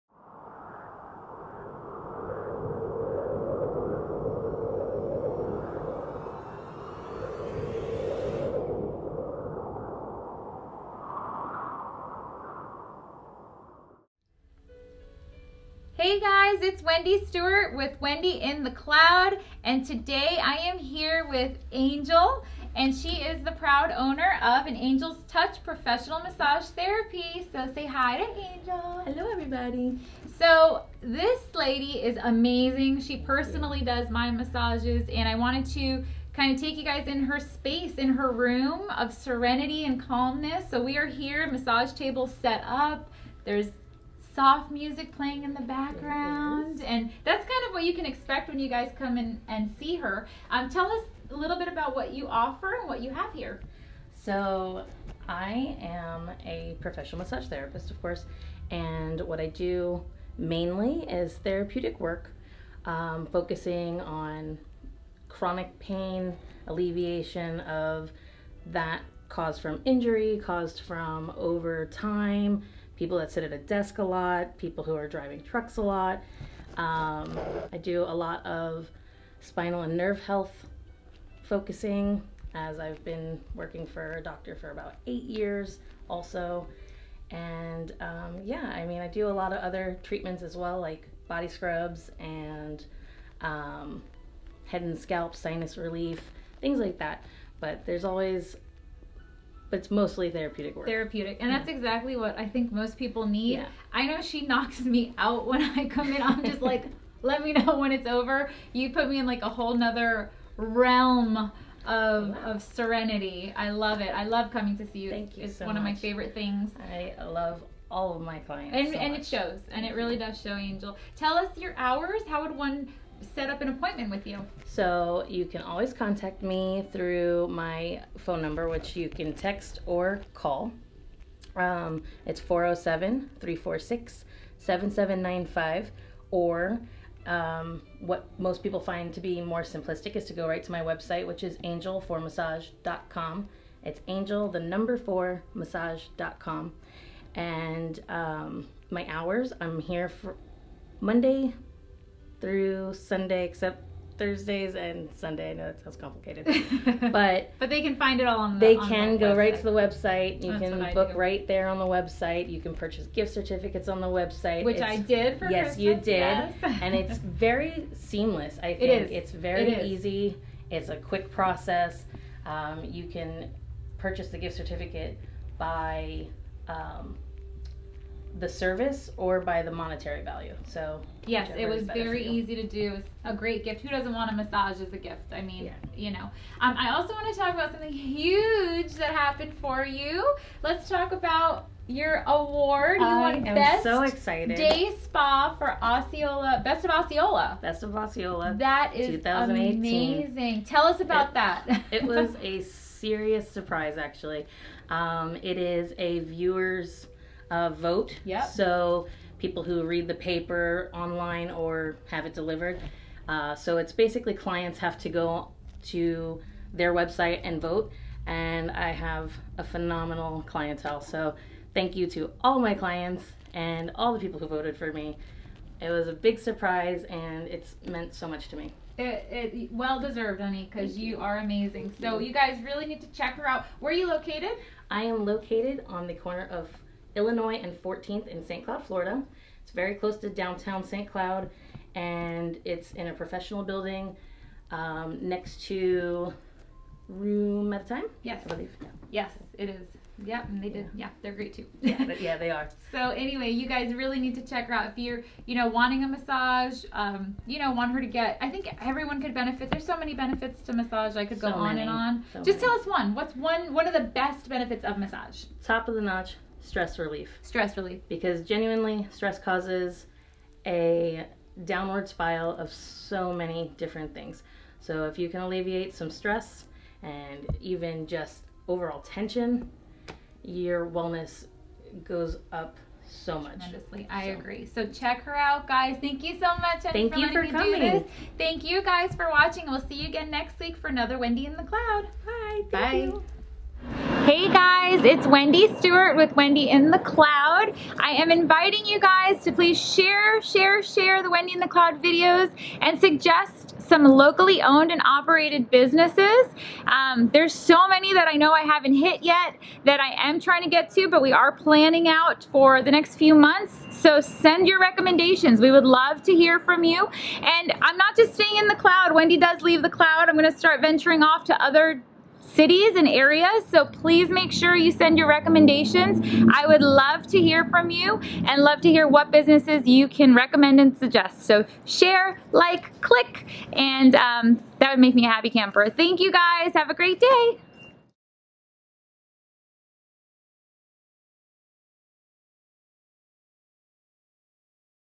Episode 5 - An Angel's Touch
Today’s Wendy in the Cloud ☁️ brought us to An Angel's Touch Professional Massage Therapy with Angel Grieco!!! Her hands are heavenly and I legit am the most relaxed while under her angel wings 😇She offers a variety of therapeutic massage as well as several other types of massage. You’ll be glad ya did!